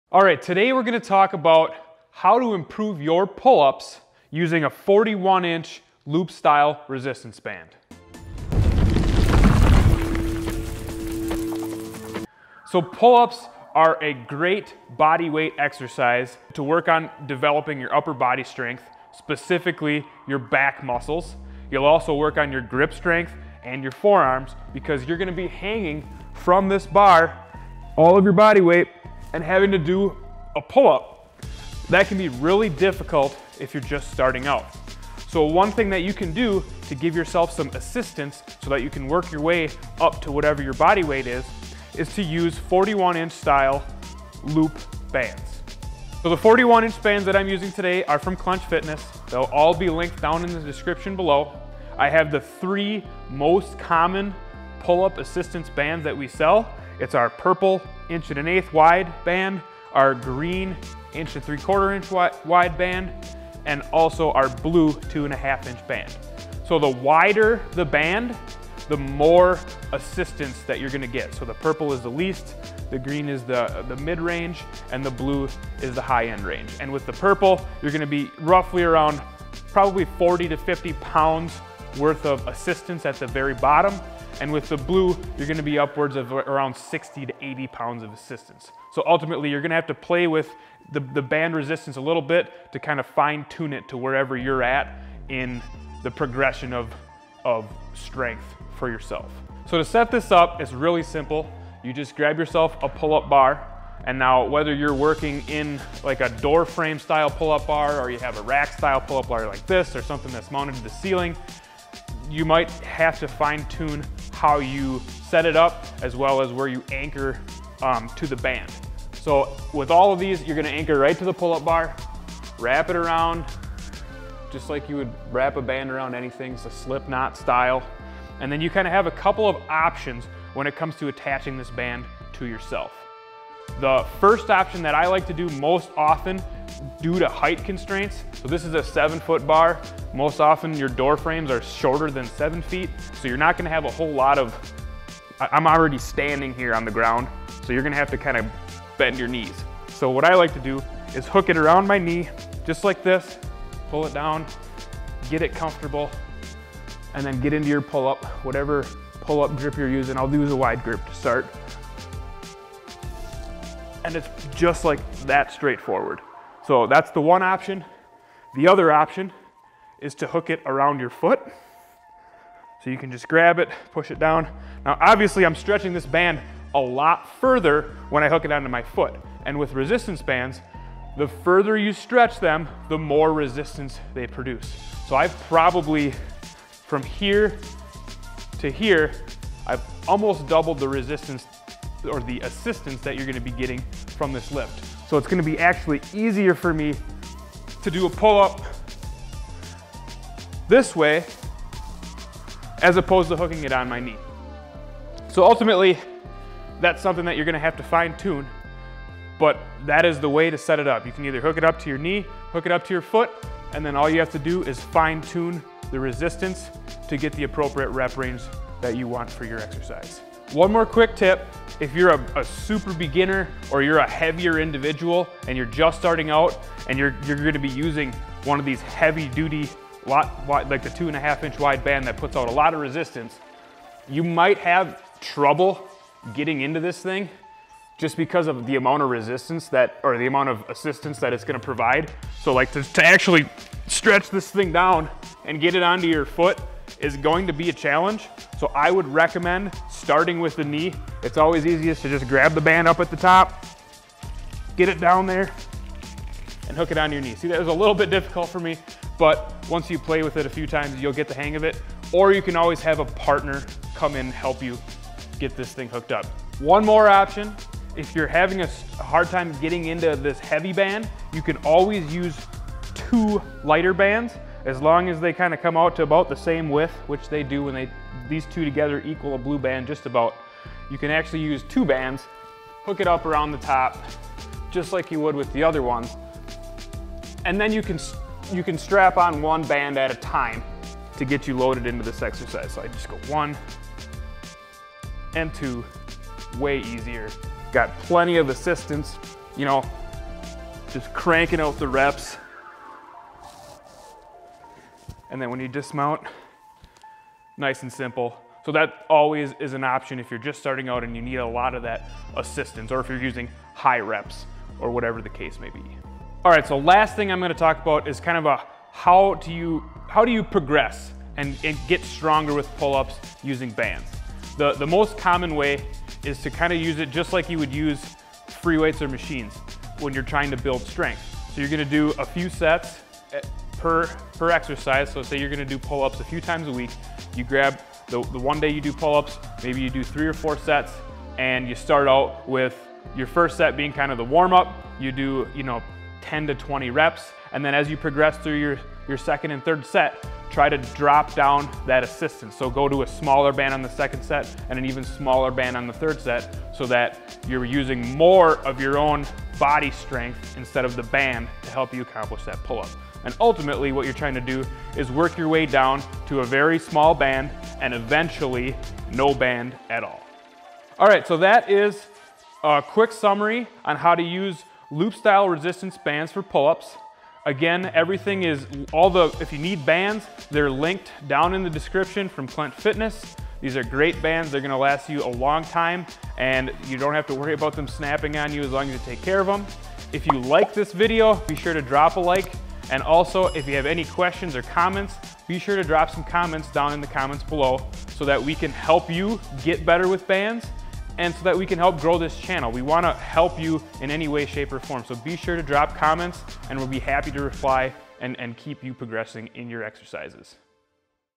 Resistance Band Pull Up - Banded Chin Up 2023
Watch this video if you need a little assistance with your pull ups or chin ups, or if you're wondering how to do a pull up if you can't do one.

We'll walk you through how to do a resistance band pull up or banded chin up so that you can start doing more pull ups today!

The concept is pretty simple.  We are going to add a 41" loop style resistance band to your pull up bar, and you're going to hook it up to your knee or feet to give you a bit of assistance so that you can complete more pull ups through the entire range of motion.

We are using 41" loop style resistance bands (a.k.a pull up bands) from Clench Fitness because they are high quality, and there are many different resistance/assistance levels available.

The most common sizes we see used are as follows:

Purple - 1.125" wide band - great for lighter people, or people who can already complete 1-5 reps of pullups.

Green - 1.75" wide band - great for moderate weight people, or people who struggle to get 1-2 pull ups

Blue - 2.5" wide band - great for heavy people, or people who can't even get 1 pull up.